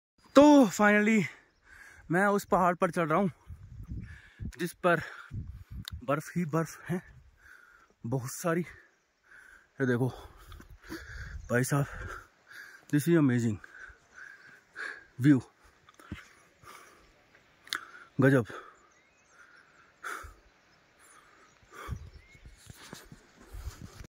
Finally at the snowline | vlog 13 | Rishabh kohl | Rupesh
Snowline amazing views of the snow. Cool and pleasent weather out there with fresh air.